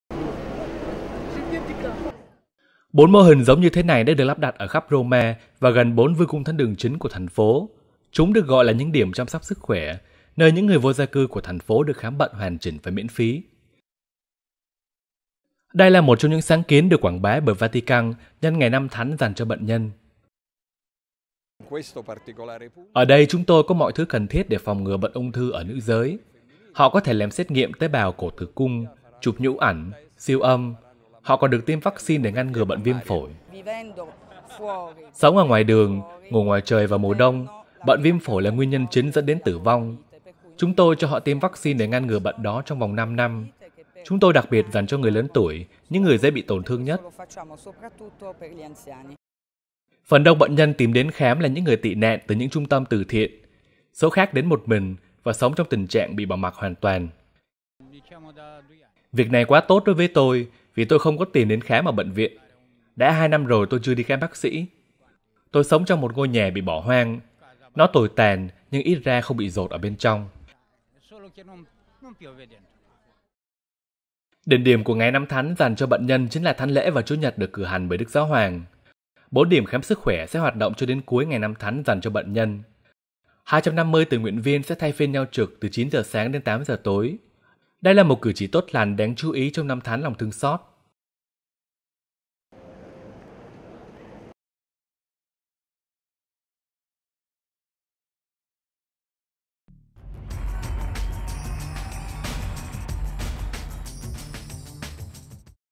Ngày Năm Thánh cho Bệnh nhân: Người vô gia cư gần Vatican được khám chữa bệnh miễn phí
Một sáng kiến kéo dài trong 4 ngày bởi Vatican nhân dịp Ngày Năm Thánh dành cho Bệnh nhân.